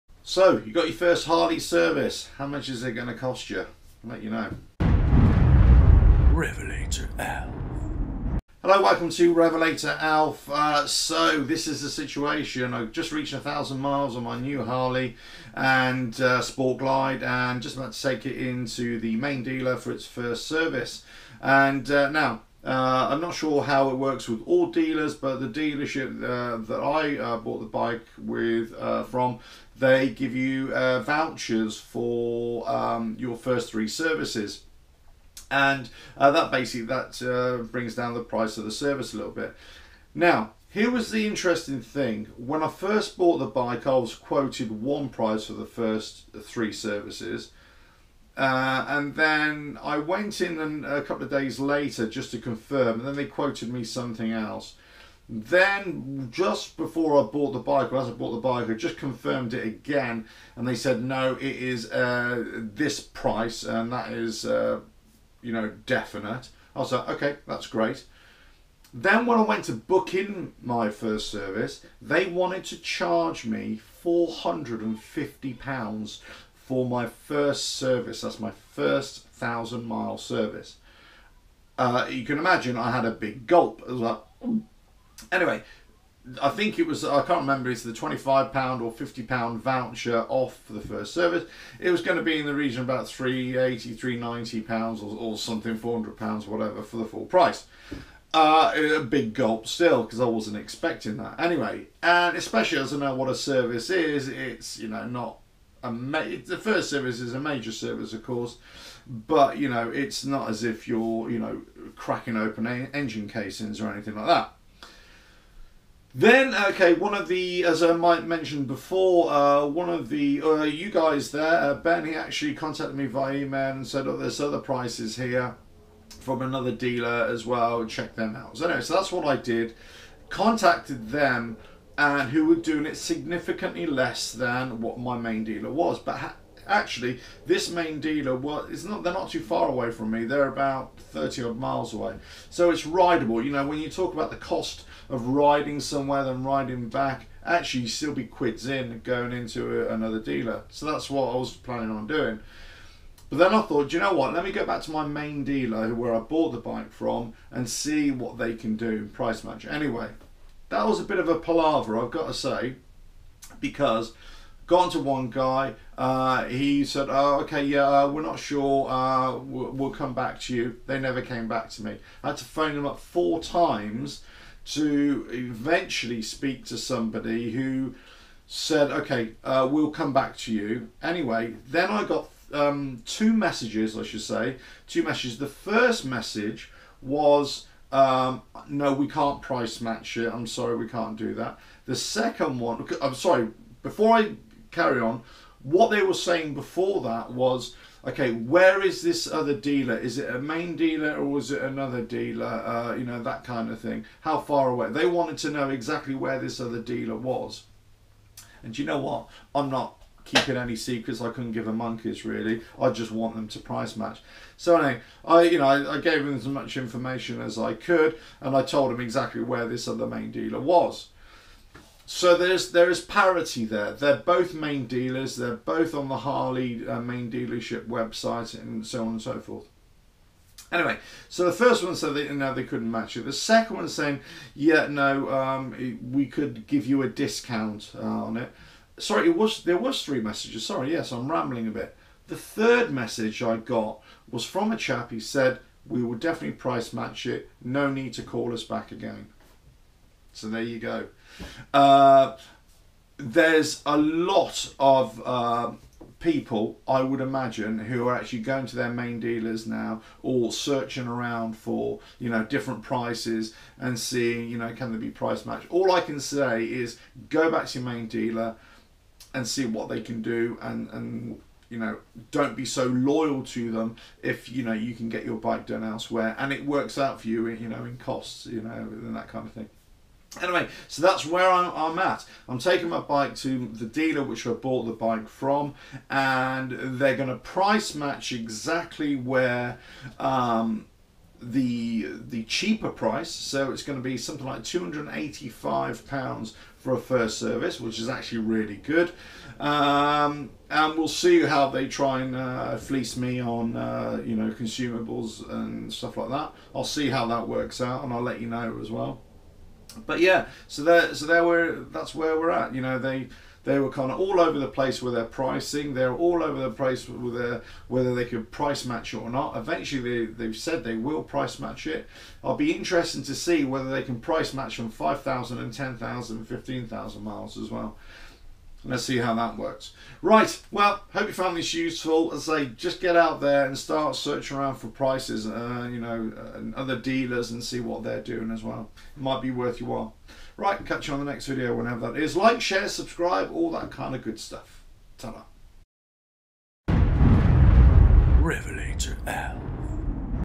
How much will my Harley servicing cost from a main dealer
How much will my Harley servicing cost from a main dealer - In this video I talk about the servicing costs from the main Harley dealer and the difference in price from other dealers or garages…or even from other main dealers too.